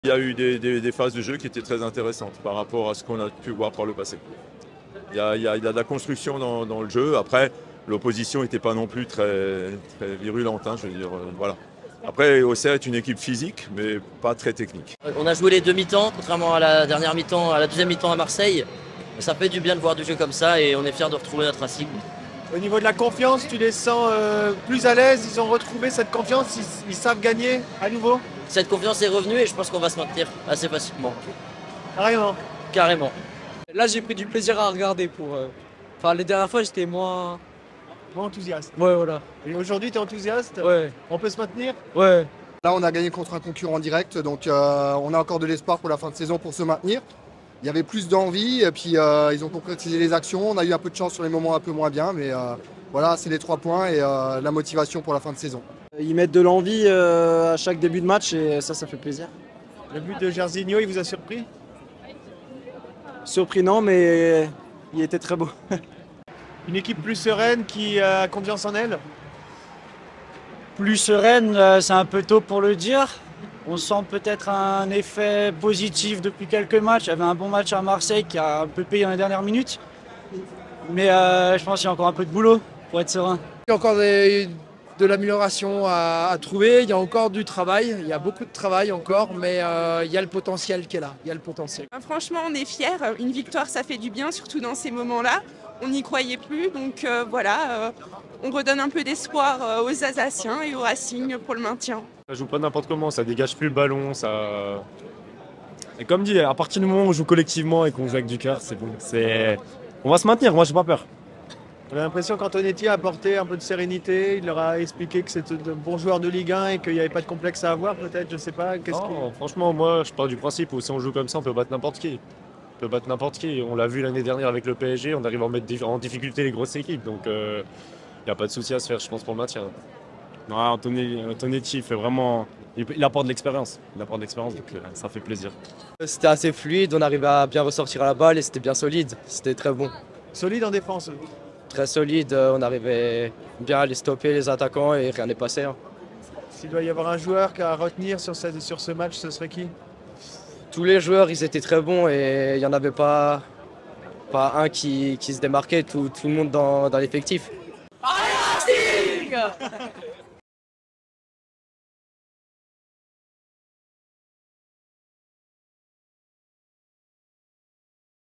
Paroles de supporters : RCSA 2 - 0 AJA
⏯️ 🎤 Une victoire à domicile face à un concurrent direct, les supporters strasbourgeois sont rassurés ! Réactions à la fin de la rencontre 🔵⚪️